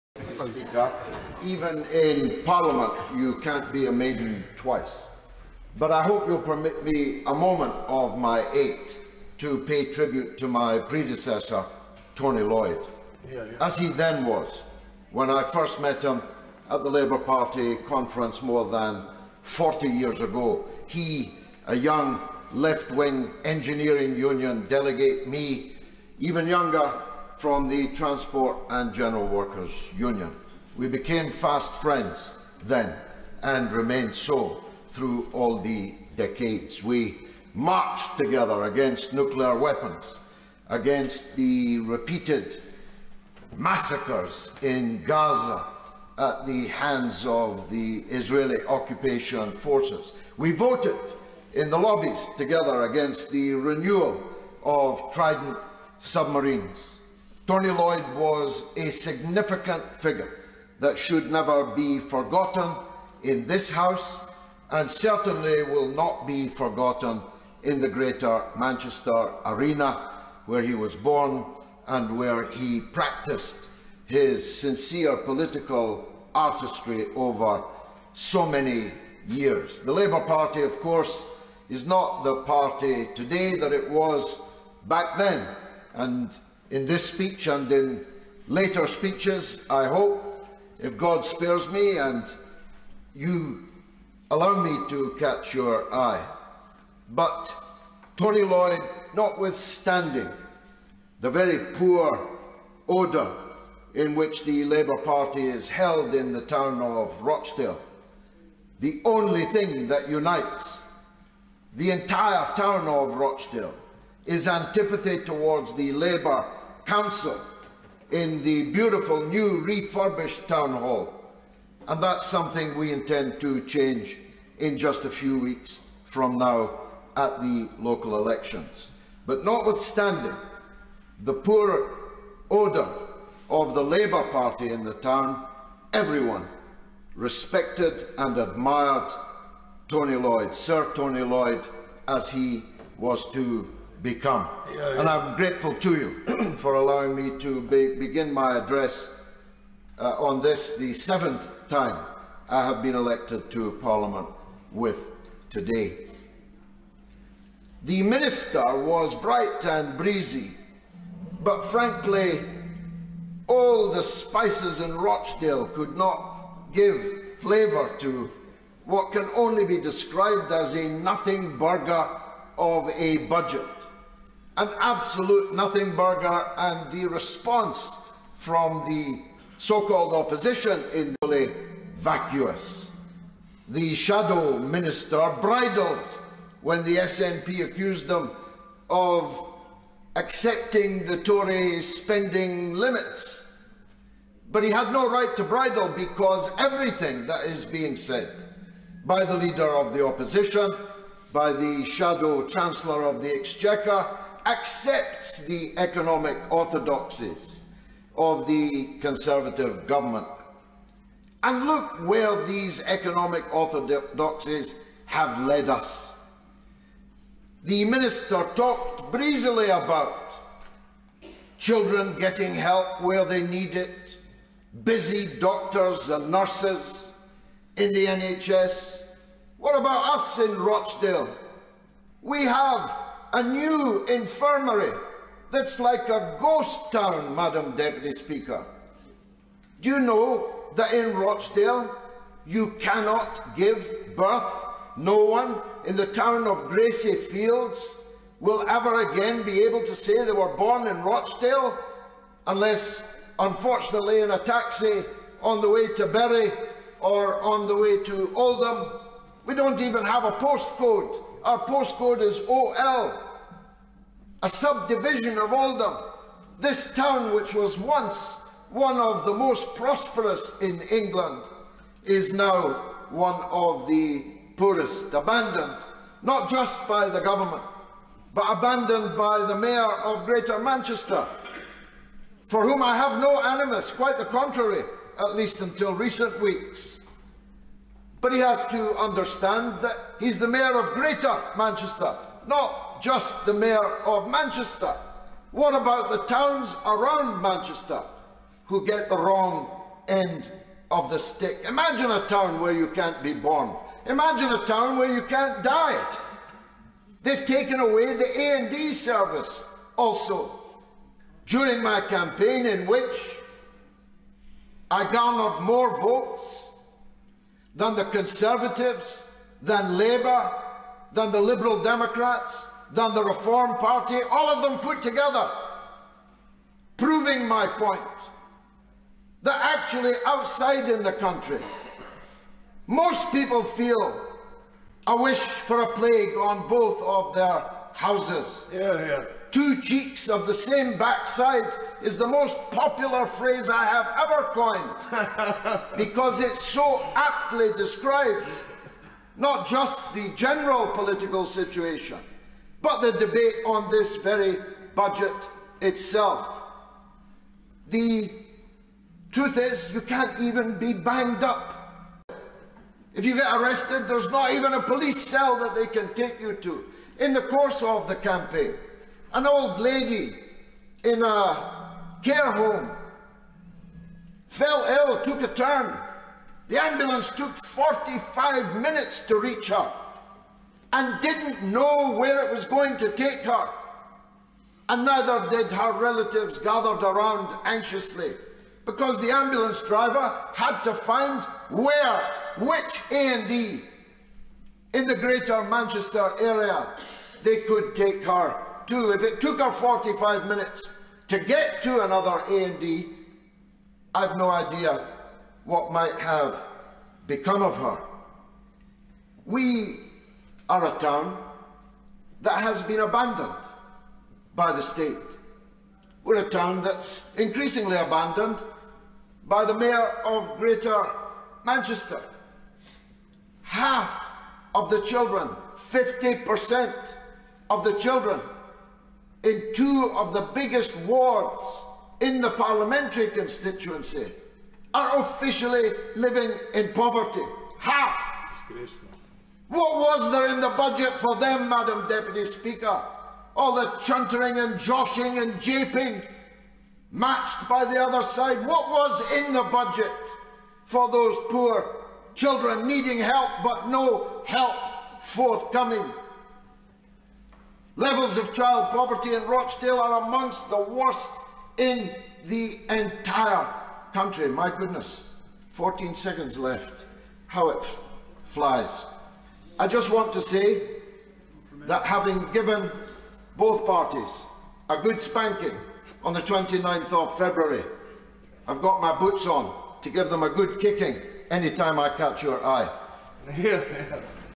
George Galloway MP Maiden Speech 2024
George Galloway MP speaks for the first time on March 12th since being elected as the member for Rochdale. He speaks of a once prosperous town that is now one of the poorest in the country, bereft even of maternity and hospital emergency services. Galloway's pledge is to make Rochdale rise again.